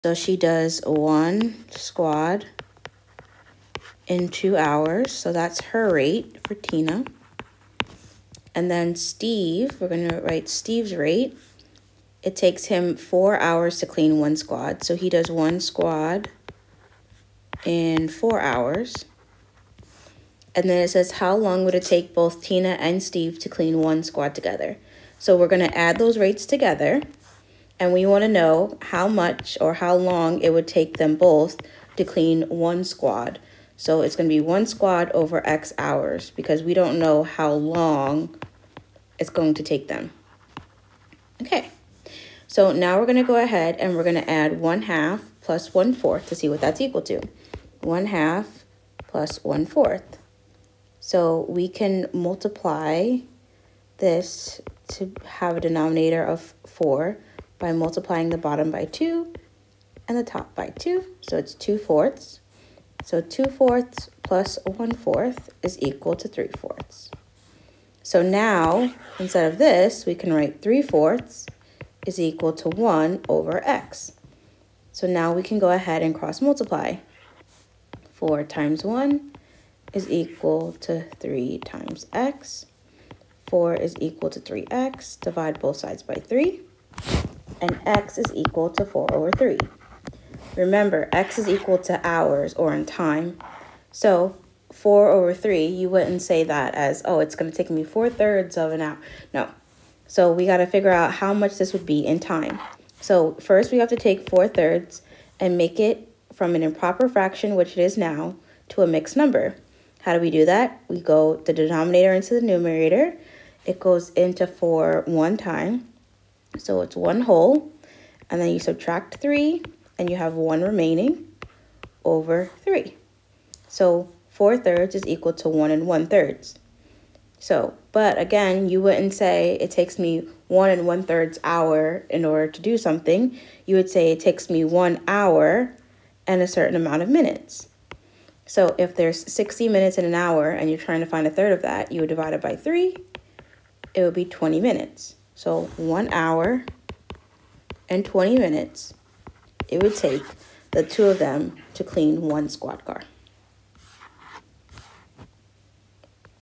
Firefighter Math Exam -If it takes Tina 2 hours to complete a task & takes Steve 4 hours...
Firefighter Math Exam questions